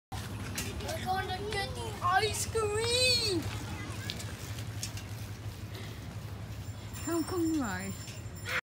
Ride to the ice cream man! I kyle
Finally he is confident to ride the bike without the training wheel